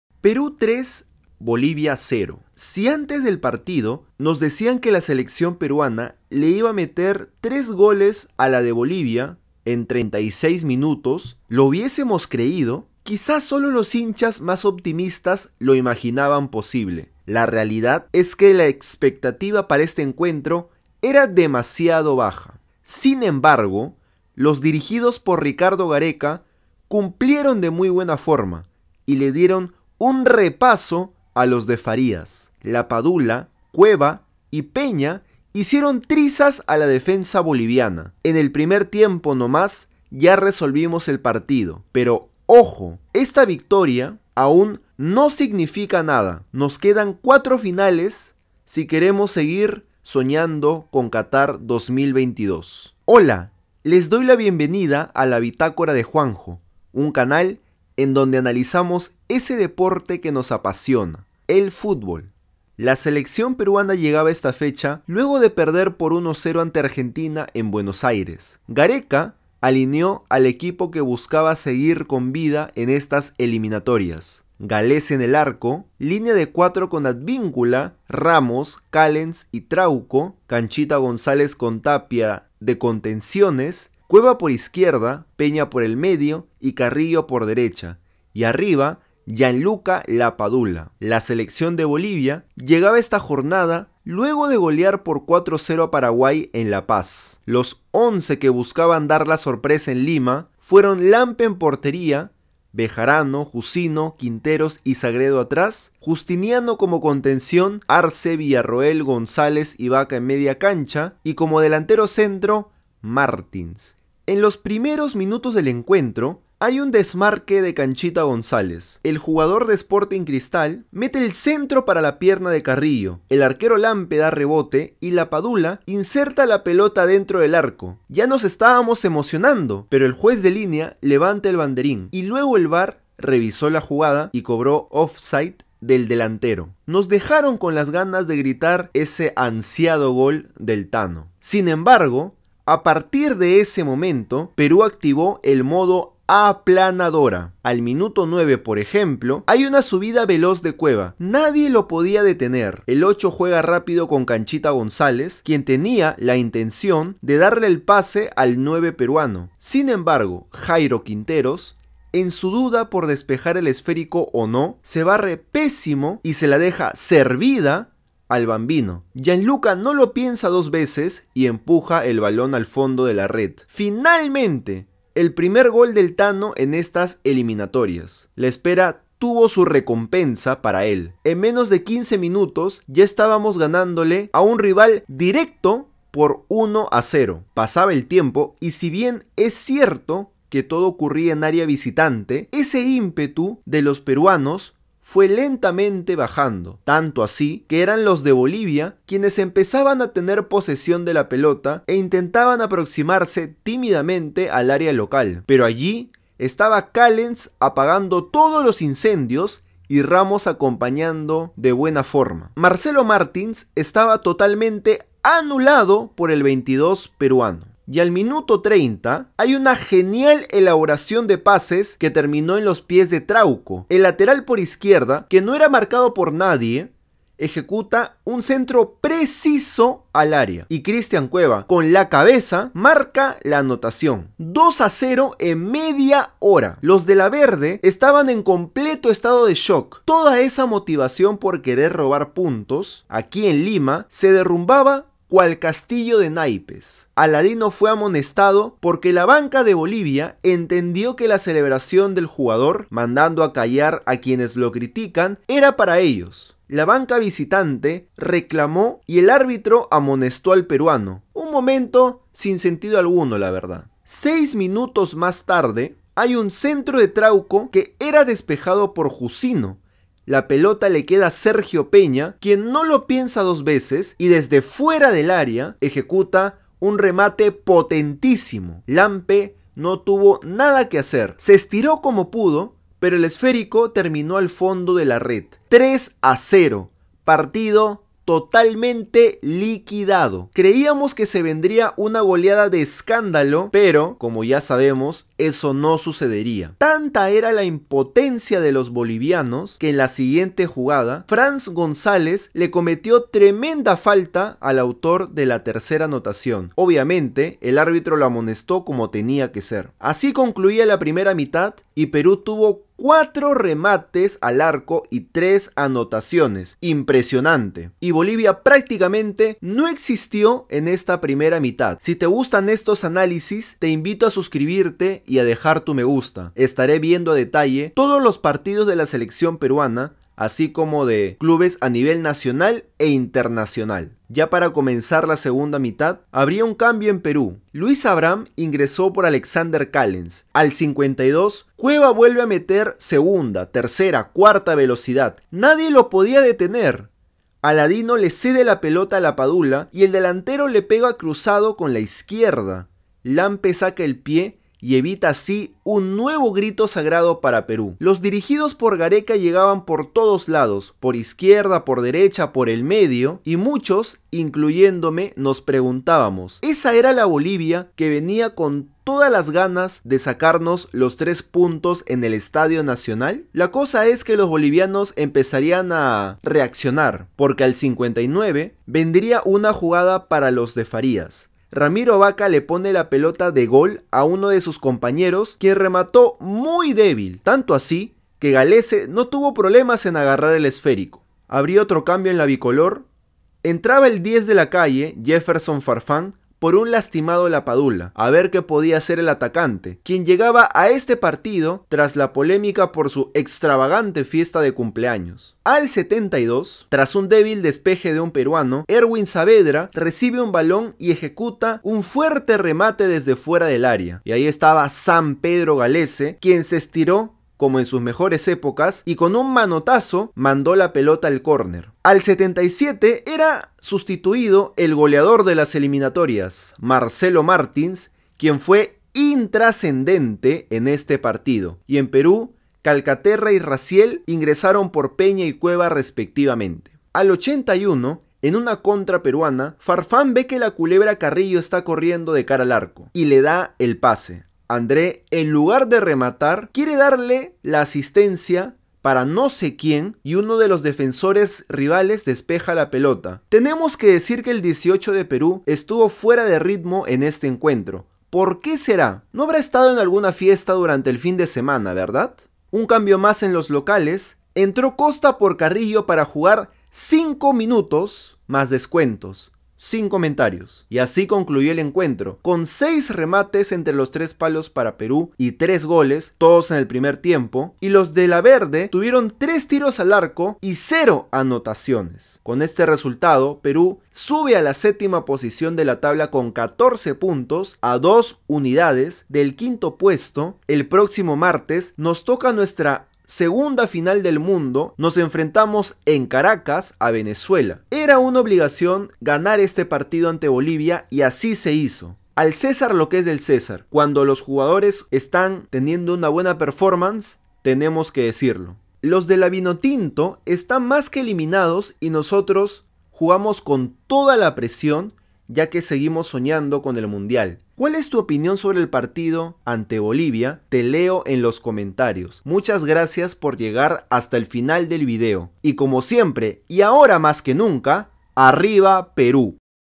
PERÚ GOLEÓ 3-0 A BOLIVIA CON GOLES DE LAPADULA, CUEVA Y PEÑA | ELIMINATORIAS QATAR 2022
En una primera mitad de fantasía, los dirigidos por Ricardo Gareca vencieron y golearon a los de Farías. La actitud avasalladora de los peruanos fue imposible de contener. El "Bambino" Lapadula por fin pudo anotar su primer tanto en este proceso rumbo a Qatar 2022. Cueva (de cabeza) y Peña sentenciaron el encuentro. Revive lo mejor de este partido aquí, en La Bitácora de Juanjo